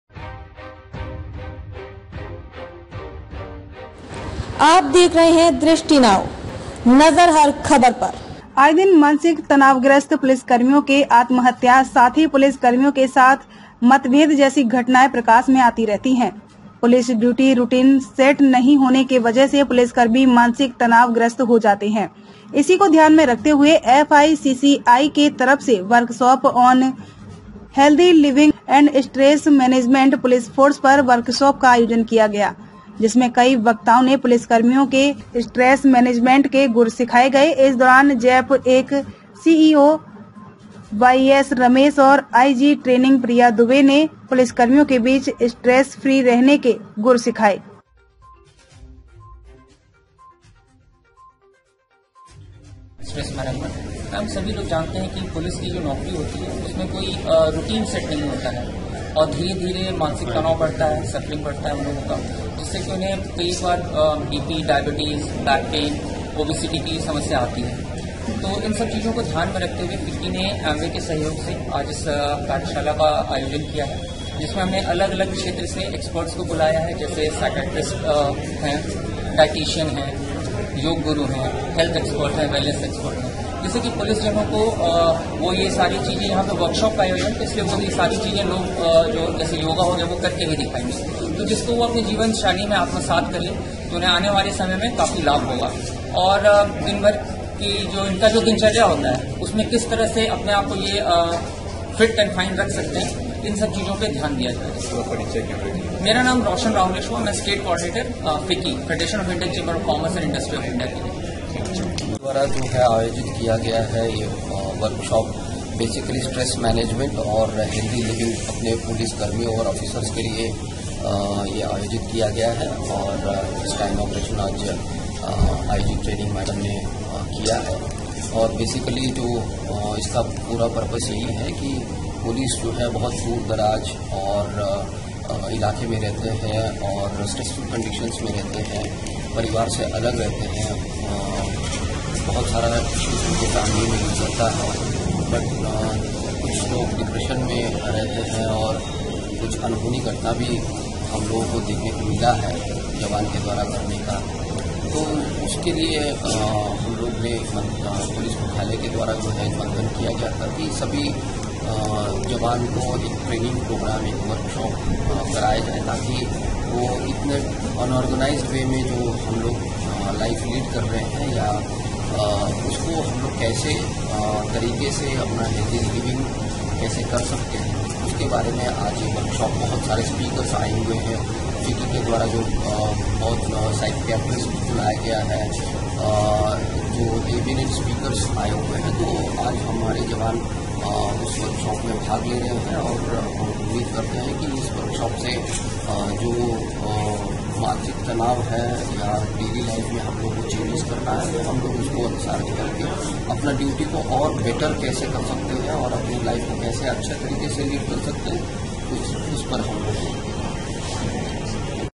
पुलिसकर्मियों के बीच हेल्दी लिविंग एंड स्ट्रेस मैनेजमेंट पर किया गया वर्कशॉप
राँची: आए दिन मानसिक तनावग्रस्त पुलिसकर्मियों के आत्महत्या, साथी पुलिस कर्मियों के साथ मतभेद जैसी घटनाएं प्रकाश में आती रहती है। पुलिस ड्यूटी रूटीन सेट नहीं होने की वजह से पुलिसकर्मी मानसिक तनाव ग्रस्त हो जाते हैं। इसी को ध्यान में रखते हुए एफ आई सी सी आई के तरफ से वर्कशॉप ऑन हेल्दी लिविंग एंड स्ट्रेस मैनेजमेंट पुलिस फोर्स पर वर्कशॉप का आयोजन किया गया। जिसमें कई वक्ताओं ने पुलिसकर्मियों में स्ट्रेस मैनेजमेंट के गुर सिखाए गए। इस दौरान जैप 1 सीईओ वाईएस रमेश और आईजी ट्रेनिंग प्रिया दुबे ने पुलिसकर्मियों के बीच स्ट्रेस फ्री रहने के गुर सिखाए।

बाइट: वाईएस रमेश co जैप 1

बाइट: प्रिया दुबे आईजी ट्रेनिंग

बाइट: संदीप मुखर्जी ऑर्गेनाइजर